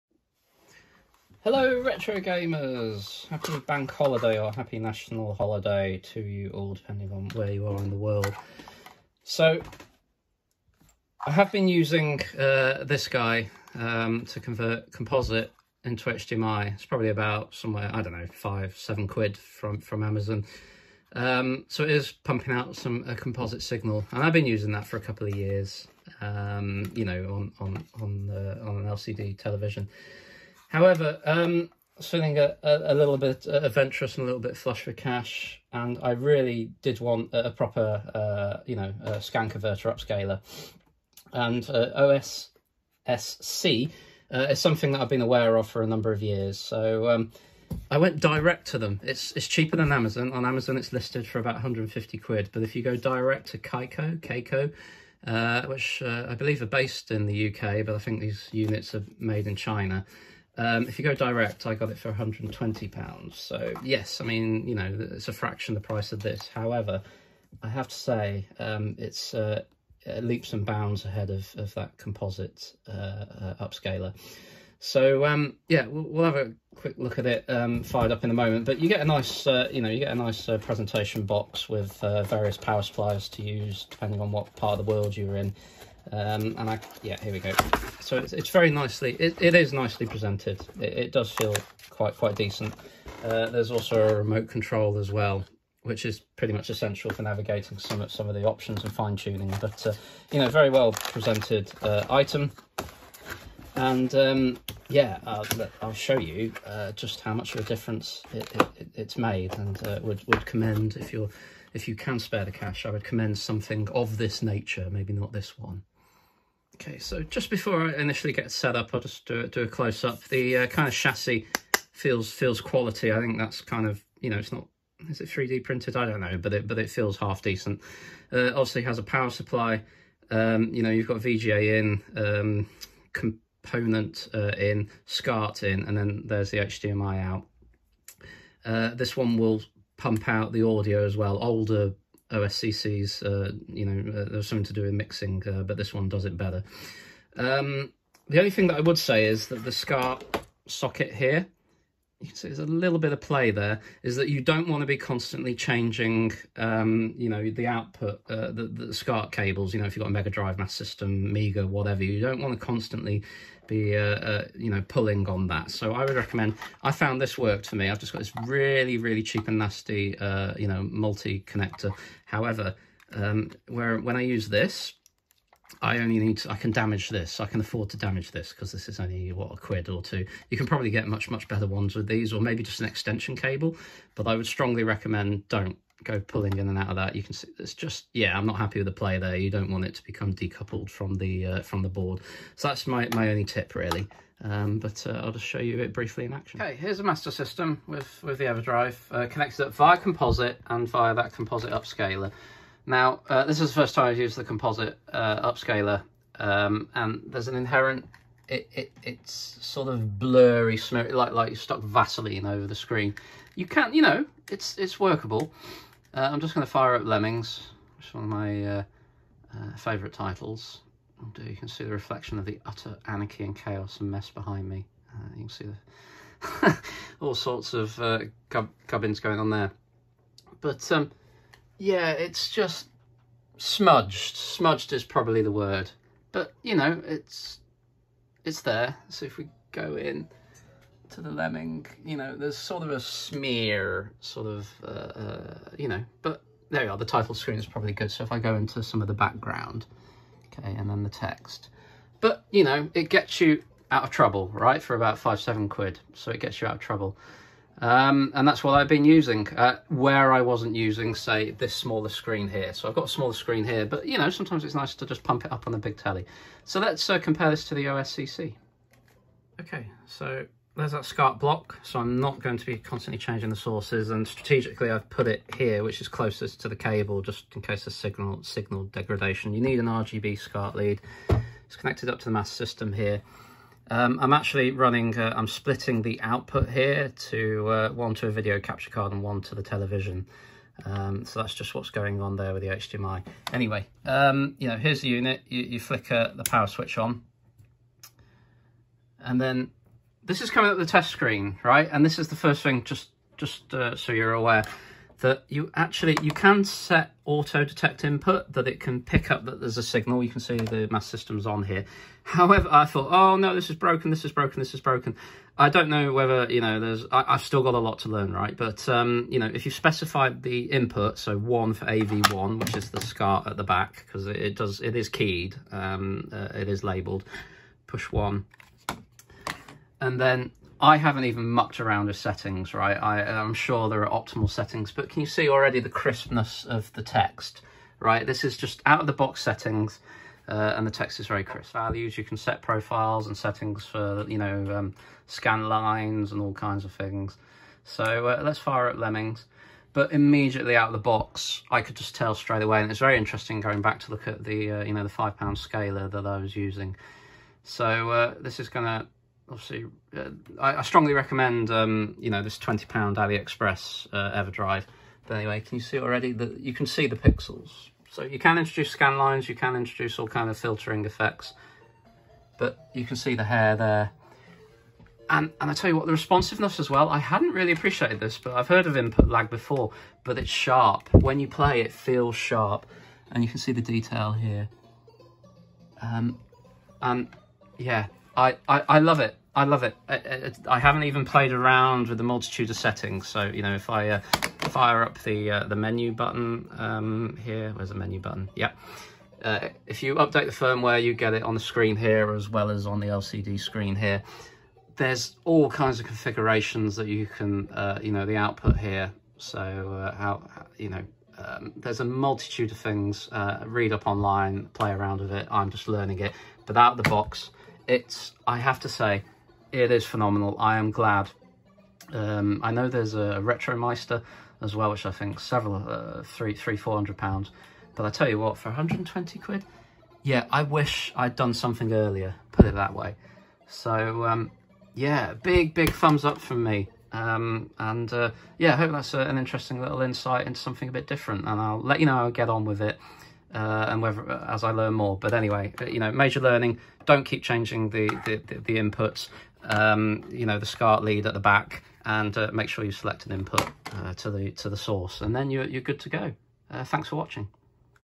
Kaico Open Source Scan Converter (OSSC) initial thoughts and review with a Master System
Had been using a cheap and cheerful composite upscaler and dug into the pockets and bought an OSSC direct from Kaico rather than Amazon. Other variants and resellers are available for OSSC but the savings compared to Aliexpress were marginal for the £120 I spent.

Here are my thoughts and initial experiences with the unit